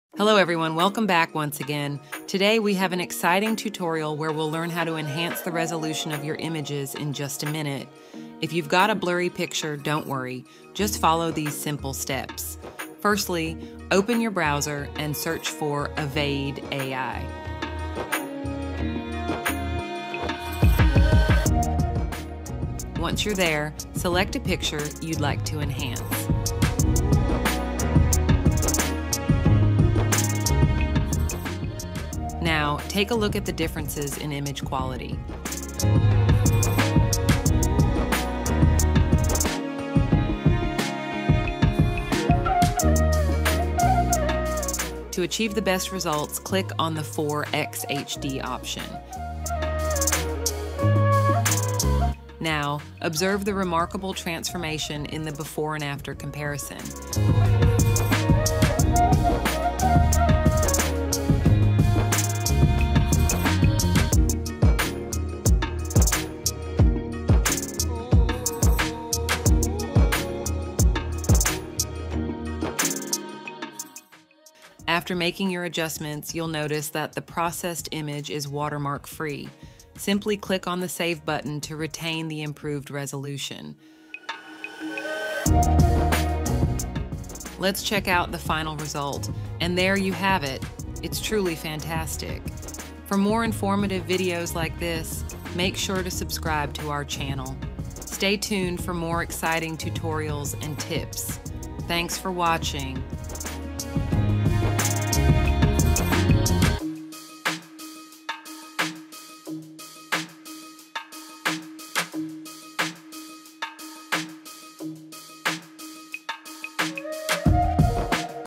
Transform Blurry to HD: Learn How to Enhance Image Resolution in Just 1 Minute! | Avaide AI Tutorial
"Revitalize your photos instantly! Discover the secrets of upgrading image resolution within a minute using Avaide AI. Say goodbye to blurry pictures and hello to stunning clarity. Watch now for a quick and easy tutorial!"